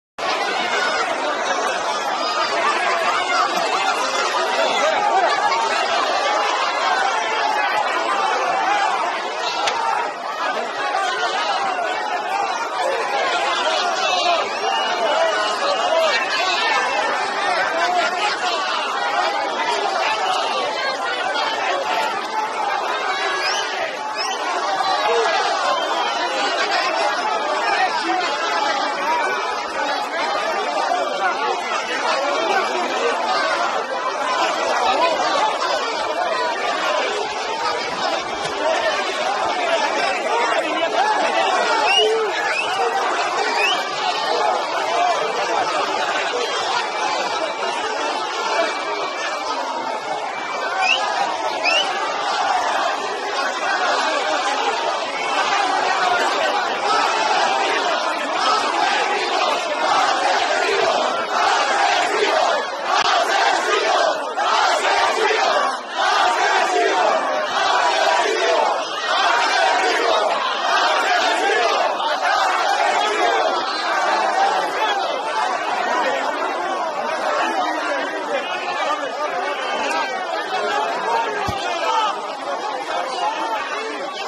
Indignados por falta de atención, vecinos de Paiporta indignados arrojaron piedras al Rey de España
🧿Indignados por la falta de atención al desastre de Paiporta, Valencia, vecinos indignados arrojaron piedras, lodo e insultaron al Rey de España

🧿Además, destrozaron el vehículo del Presidente PedroSánchez quien tuvo que huir del lugar